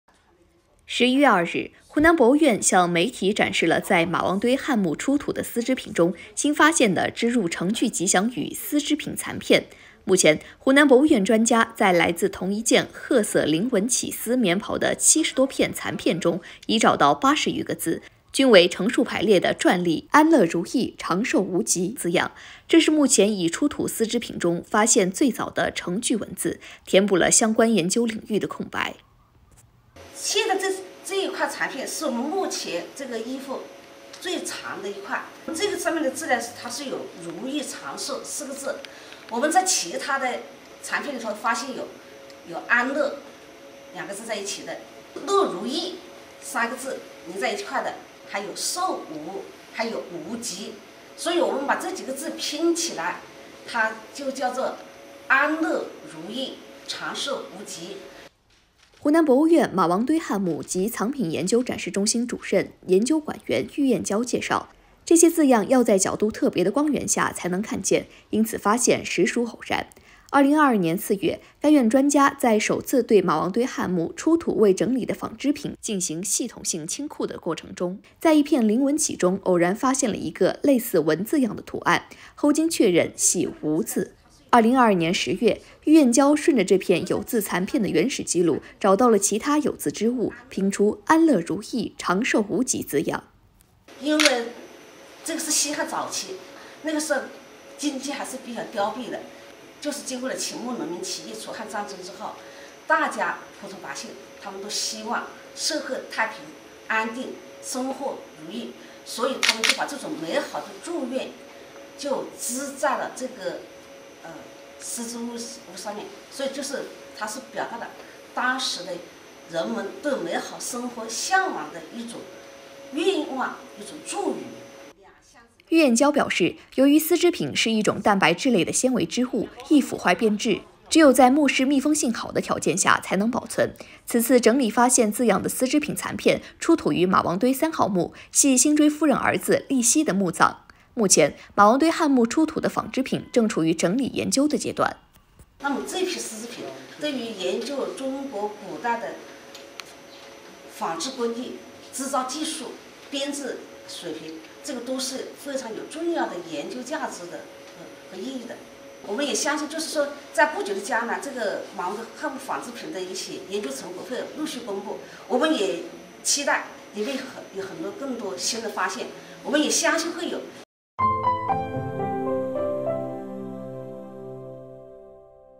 马王堆汉墓出土丝织品现成句吉祥语 专家称有重要价值
11月2日，湖南博物院向媒体展示了在马王堆汉墓出土的丝织品中，新发现的织入成句吉祥语丝织品残片。目前，湖南博物院专家在来自同一件褐色菱纹绮丝绵袍的70多片残片中，已找到80余个字，均为成竖排列的篆隶“安乐如意，长寿无极”字样。这是目前已出土丝织品中发现最早的成句文字，填补了相关研究领域的空白。